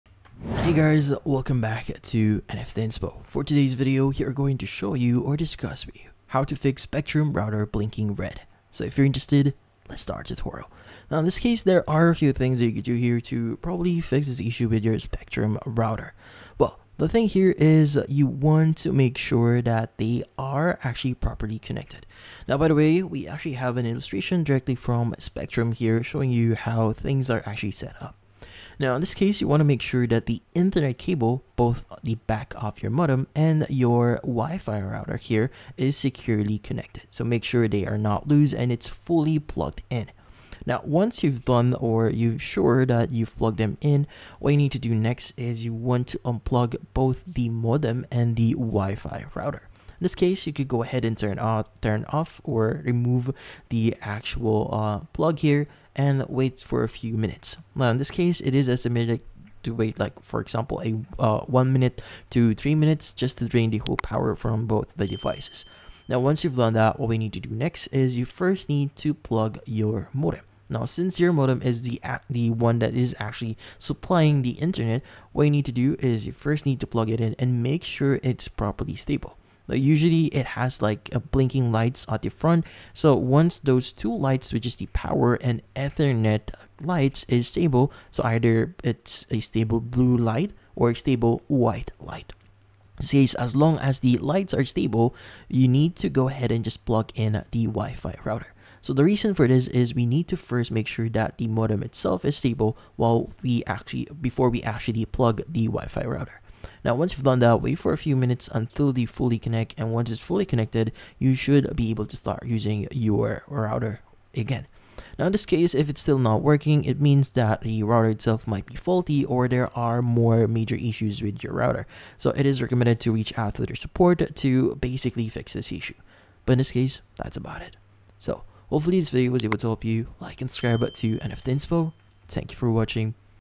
How To Fix Spectrum Router Blinking Red
If you want to be able to fix your Spectrum router blinking red, then this video will be perfect for you!